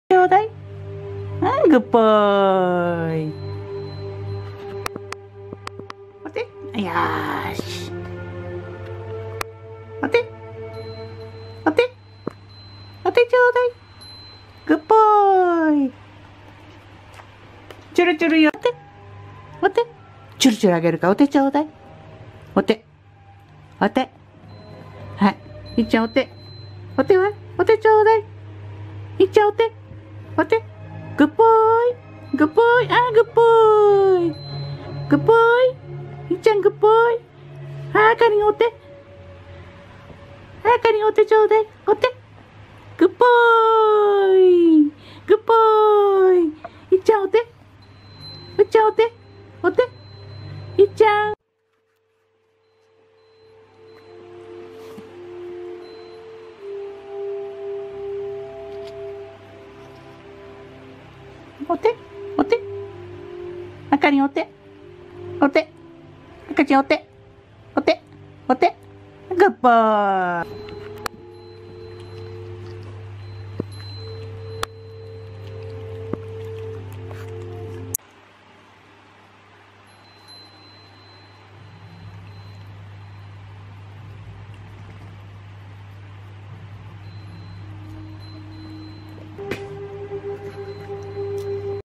お手する猫･絶対しない猫・ちょっことする猫
我家の子供猫は亡くなったパパ猫のまねをして昔からお手が出来る。
茶色のママ猫は１５歳になるが、いままで一度もしたことがない。